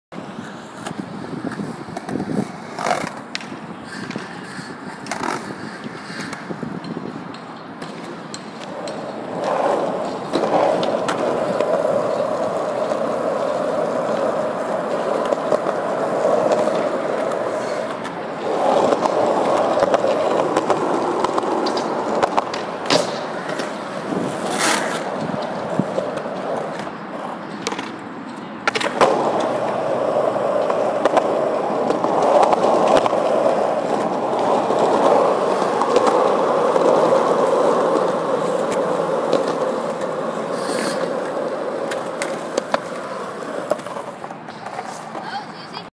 Rhodes skate line
Skate line at Rhodes skatepark in boise idaho.